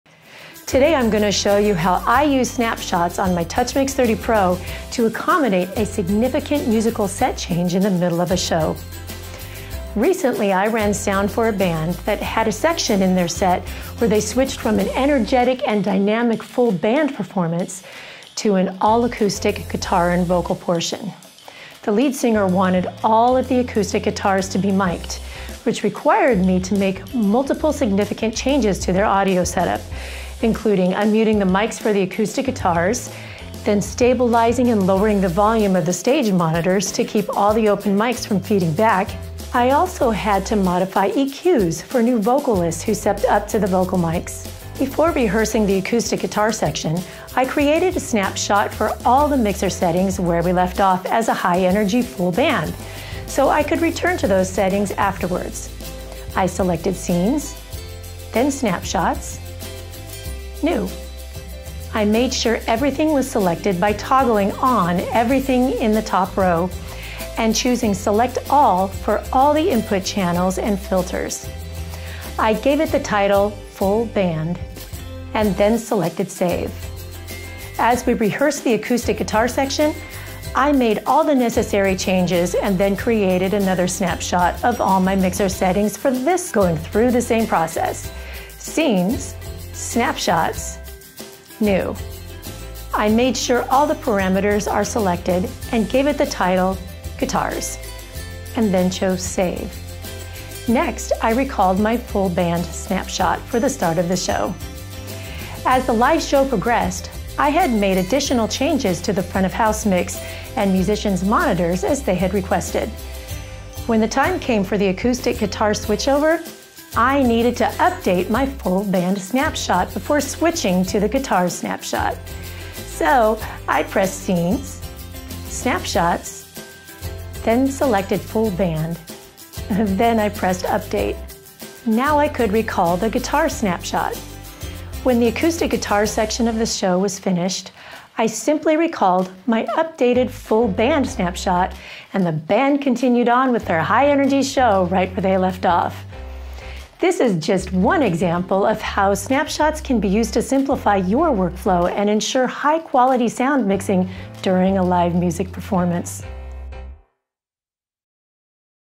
Using TouchMix Snapshots for Set Changes - Tech Talk
Learn how to use Snapshots on the TouchMix to quickly changes mixer settings on the fly for set changes.